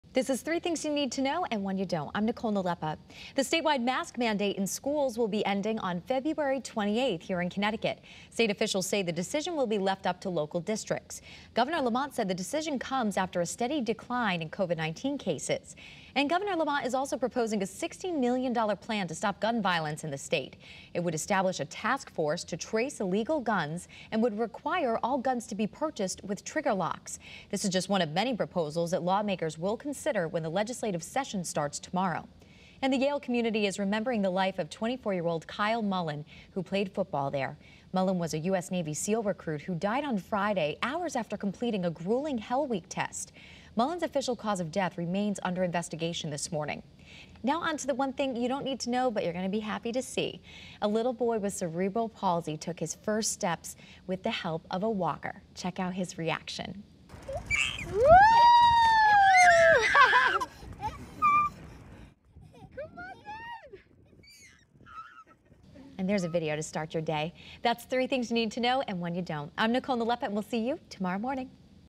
3 Things You Need to Know, 1 You Don't: February 8th
The end of the statewide mask mandate, proposal to end gun violence, a former Yale football player dies after training, and a Trending Now to make you smile!